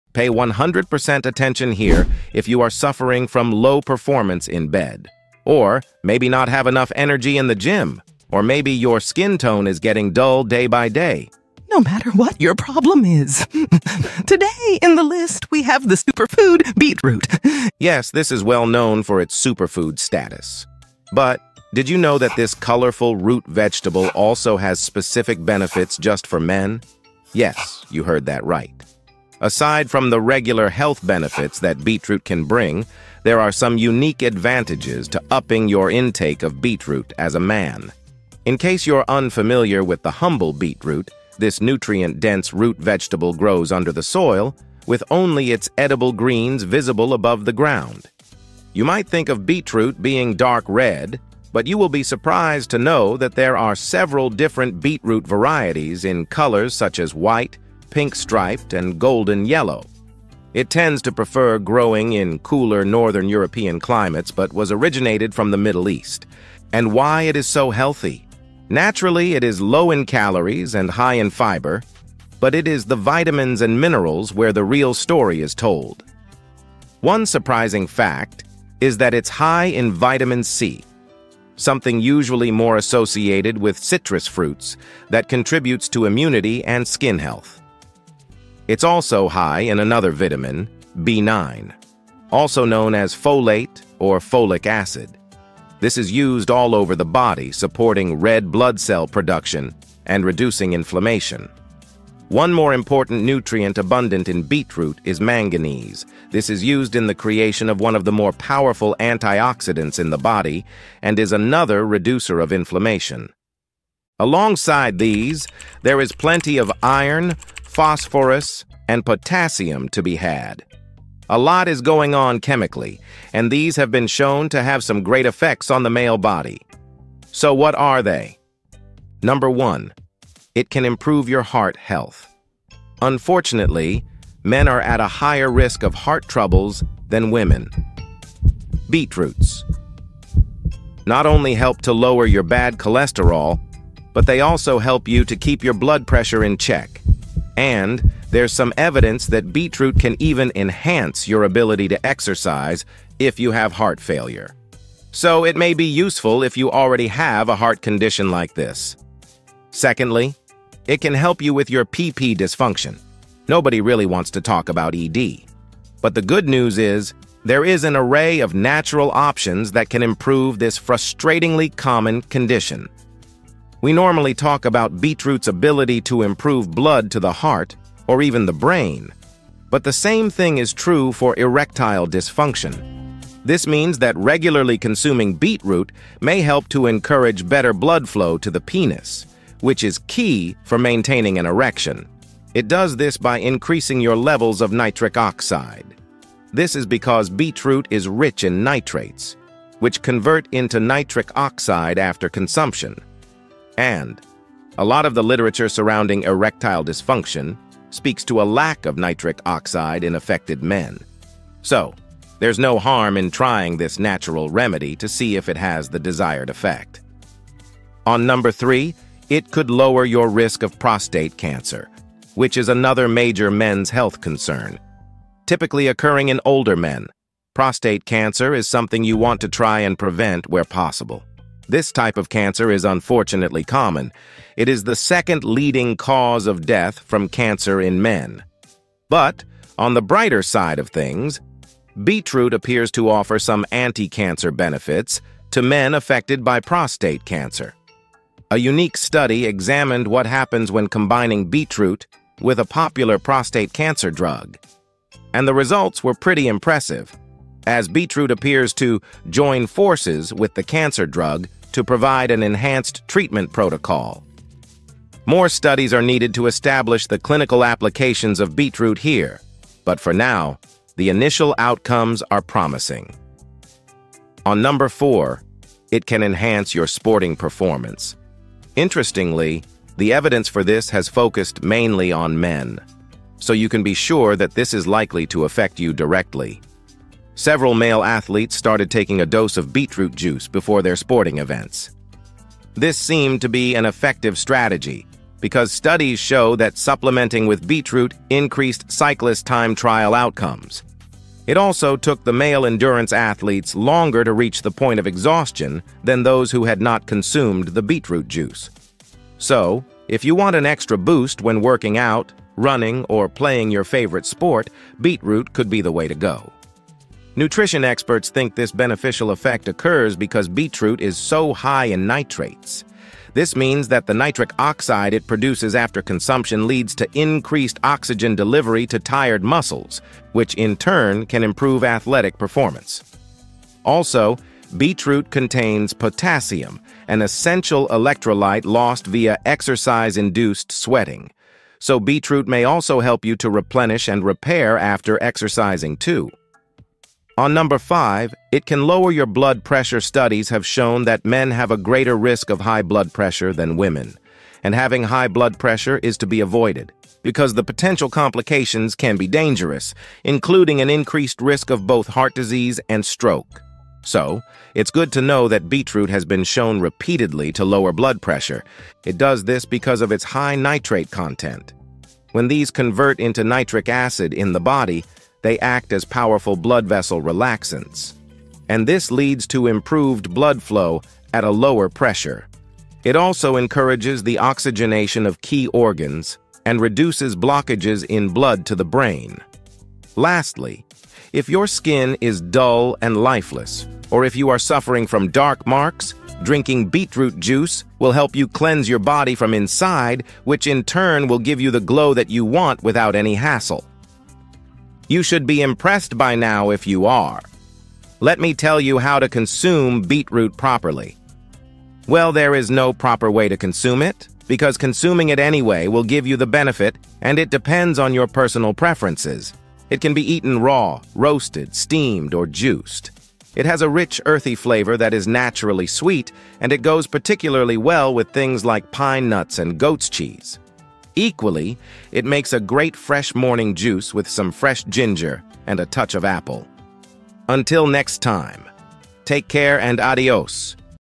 Why Men Should Drink Beetroot Juice Every Day
If you are Suffering from low performance in Bed. Or maybe not have enough energy in the Gym, or maybe your Skin tone is getting dull day by day.
No matter what your problem is. Today in the list we have the superfood Beetroot and how helpful is Beetroot juice benefits for male.